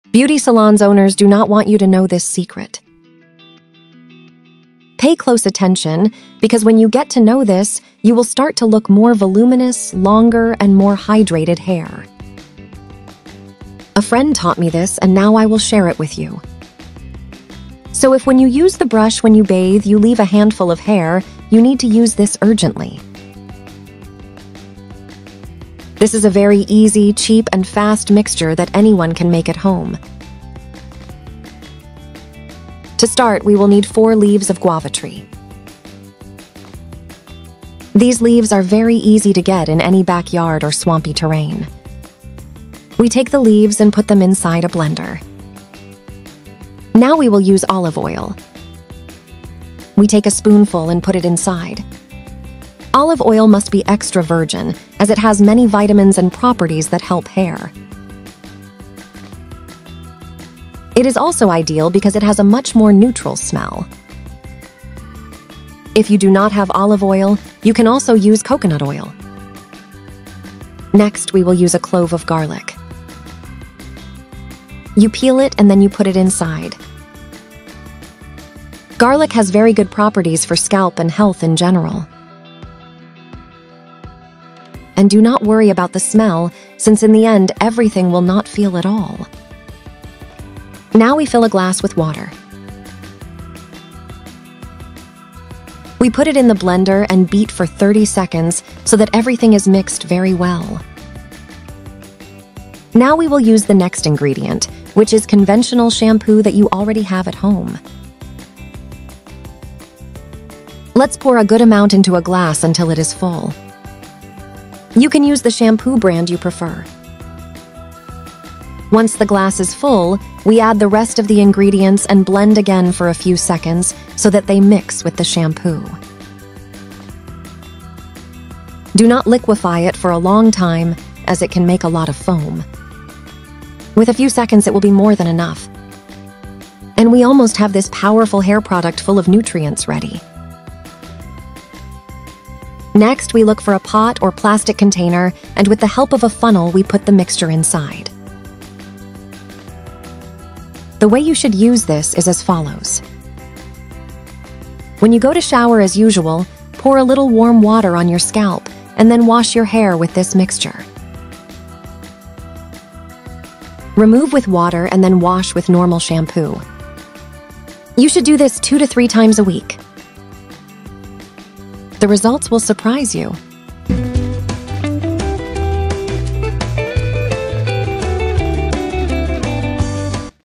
Beauty salons owners don't want you to know this
In this exciting video, we unveil a natural secret to achieving healthy and beautiful hair: the combination of guava leaves and garlic. If you're looking for a natural solution to strengthen your hair, stimulate growth, and prevent hair loss, you've come to the right place!

In this video, we'll guide you through the preparation of this potent mixture and show you the correct way to apply it for astonishing results. Discover how these common ingredients can effectively transform your hair.

Remember:

Give the video a thumbs up if you find these tips helpful.
Don't wait any longer to have naturally beautiful and healthy hair! Follow our instructions and start experiencing the benefits of guava leaves and garlic on your hair today. Your mane will thank you!